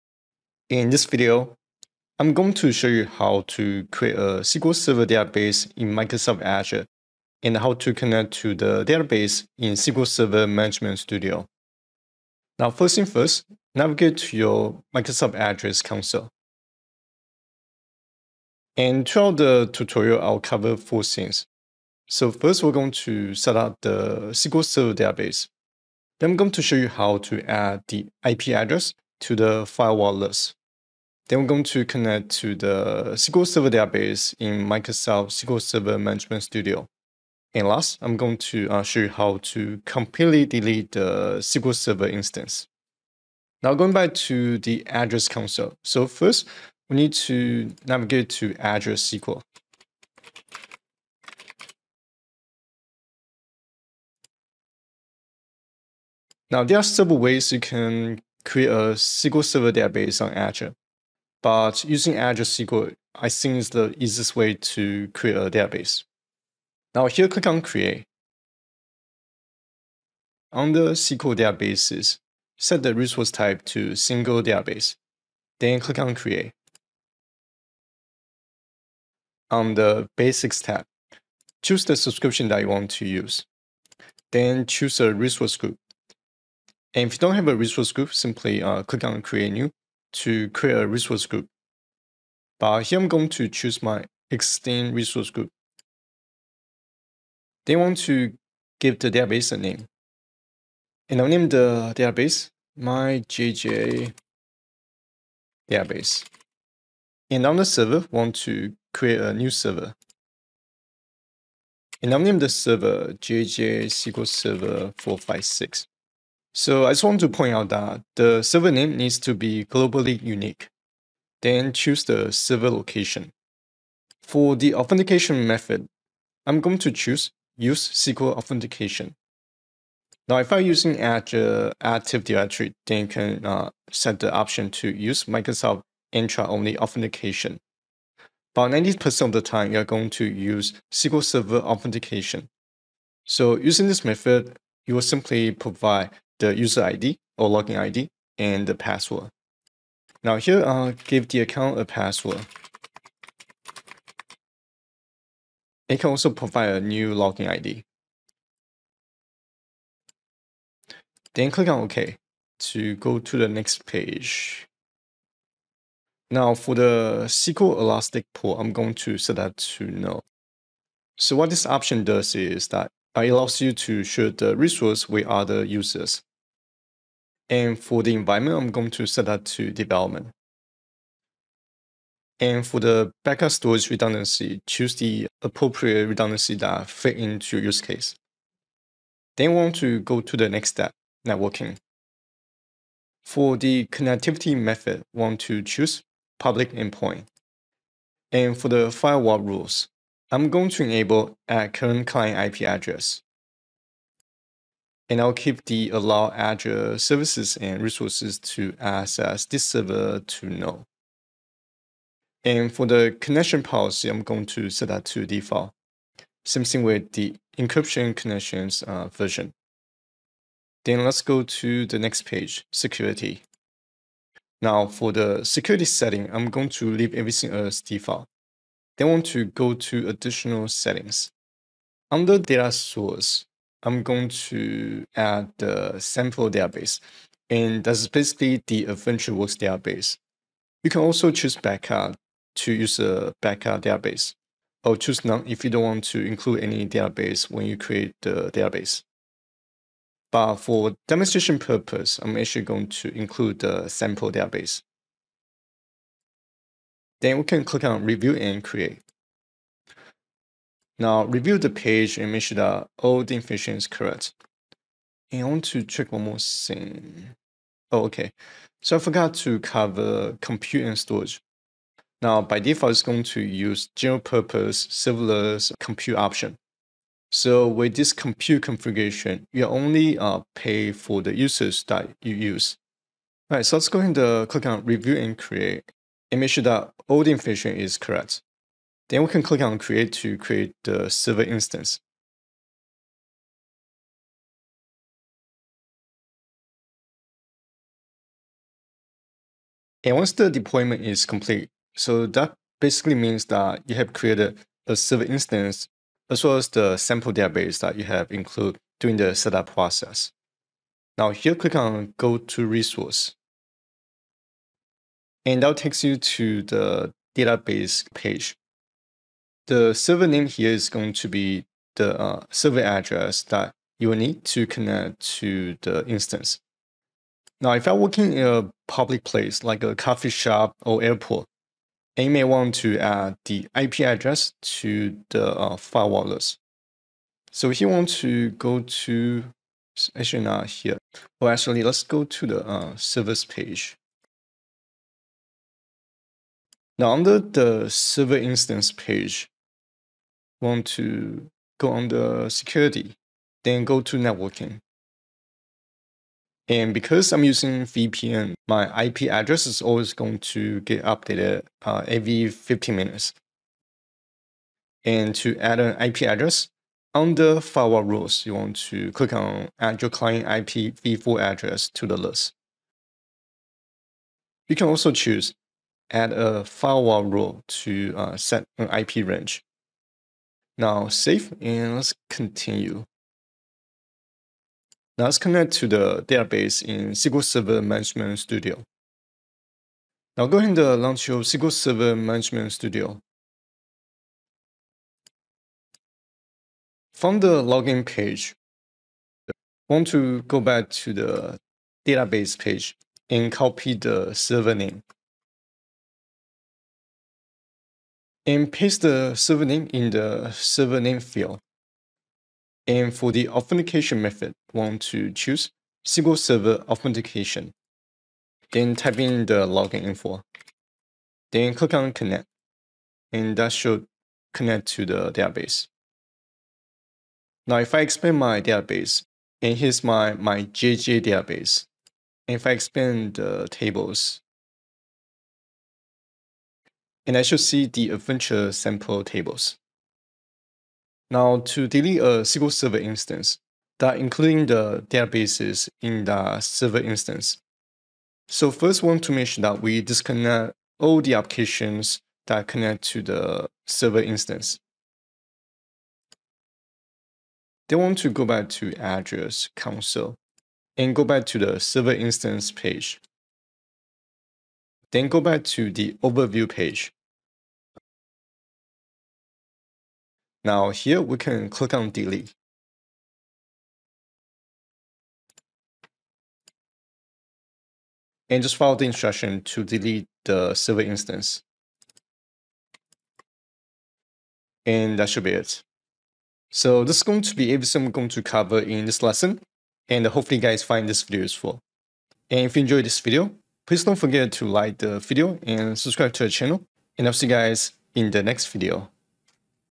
Set Up SQL Server Database In Azure & Connect To The Database With SSMS
Microsoft Azure SQL being one of the most popular Azure products today, is a must know skill for anyone who works with Azure. In this video, I will walk you through how to 1) create a SQL Server instance in Microsoft Azure, 2) create a SQL database in Microsoft Azure, and 3) how to connect to an Azure SQL database in SQL Server Management Studio.

00:00 - Set up SQL Server database in Azure
06:05 - How to add an IP address to the IP allowed address
06:47 - Connect to the SQL Server database in Microsoft SQL Server Management Studio
07:21 - Delete a SQL Server instance